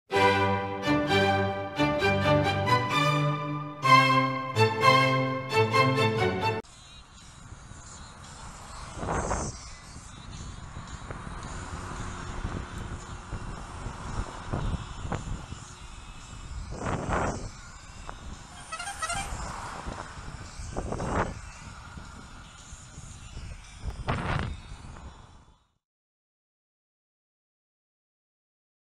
Tour de France car?
The horn on this car took me by surprise! I was looking over my shoulder for my friend who was some way behind.